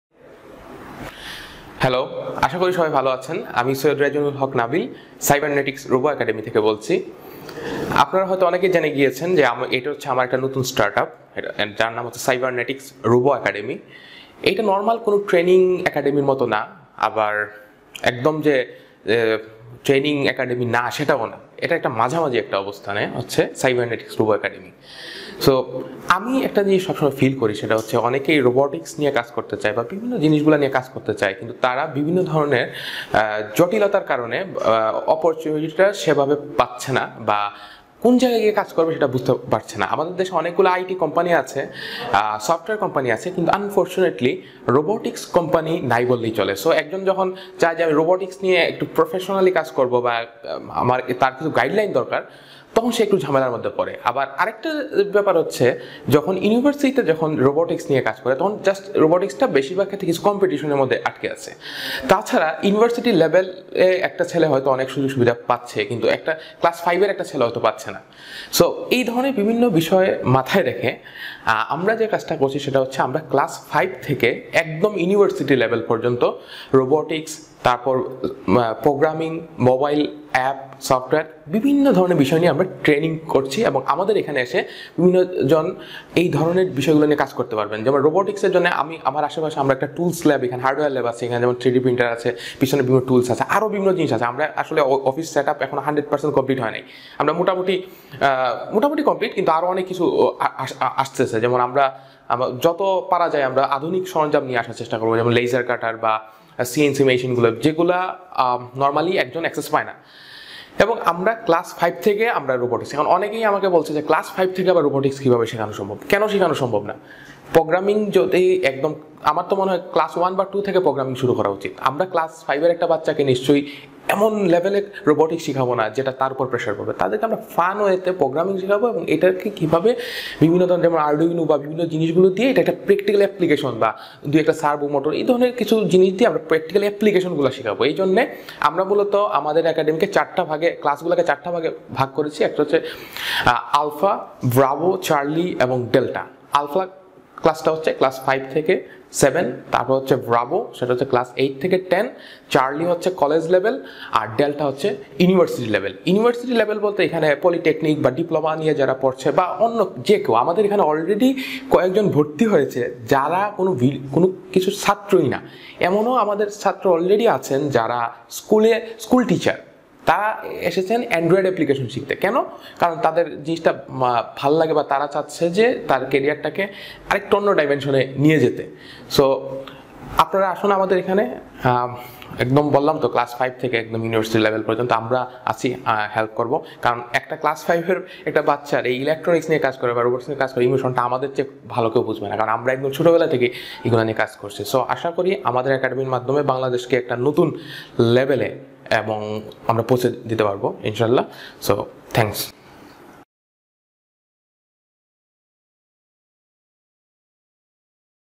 Cybernetics Robo Academy, Sylhet, Bangladesh -- Class 5 to University-- Our New Startup
Khaled Hussain , CEO
Syed Razwanul Haque Nabil , CTO

Cybernetics Robo Academy এ আমরা ৫ম শ্রেণী থেকে বিশ্ববিদ্যালয় লেভেল পর্যন্ত রোবটিকস, প্রোগ্রামিং,  ড্রোন, মোবাইল এ্যাপ এর কোর্স করিয়ে থাকি। এটা কোন প্রচলিত ট্রেনিং সেন্টার না। আমরা চাই যাদের সত্যিকারের আগ্রহ আছে তারা যেন সুযোগের অভাবে পিছিয়ে না থাকে।